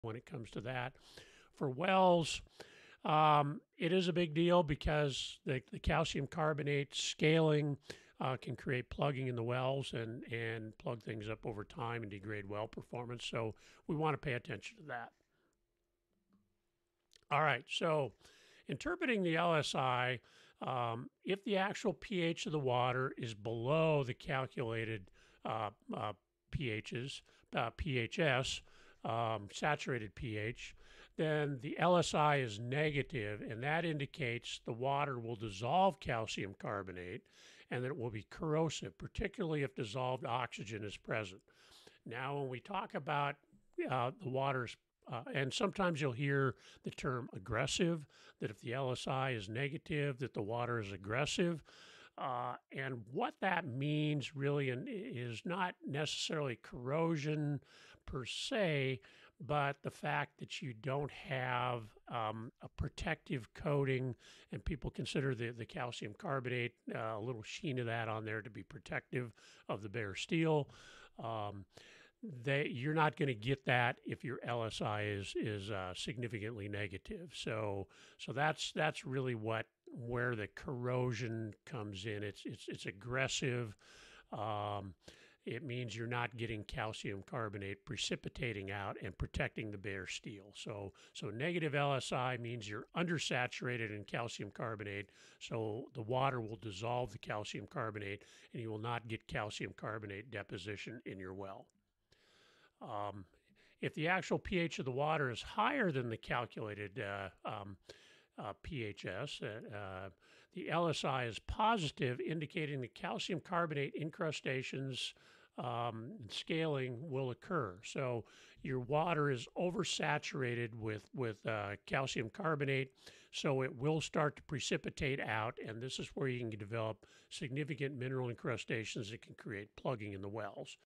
Lagelier Saturation Index Calculation
Summary content from Groundwater Talk Live explaining how the Langelier Saturation Index (LSI) is calculated.